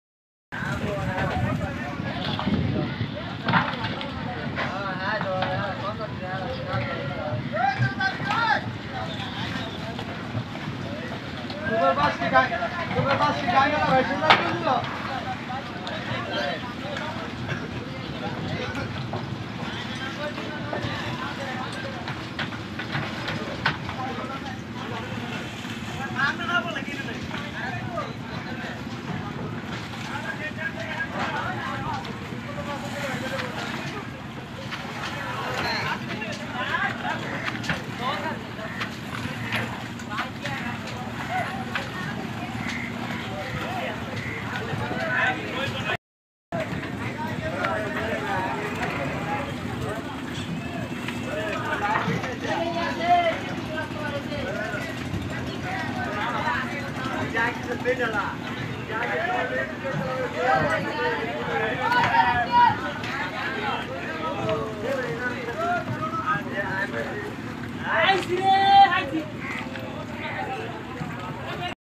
The condition of Vaidyer Bazar Kheyaghat in Sonargaon upazila of Narayanganj district of খেয়াঘাট
The condition of Vaidyer Bazar Kheyaghat in Sonargaon upazila of Narayanganj district of Bangladesh is not the same as before. The old historical ferry is lost for the evil people. In this market in the morning and afternoon there is a fish market. This market has connections with different areas jonaki news tv jonaki tv live video camera allow drivers were  jonakinewstv.net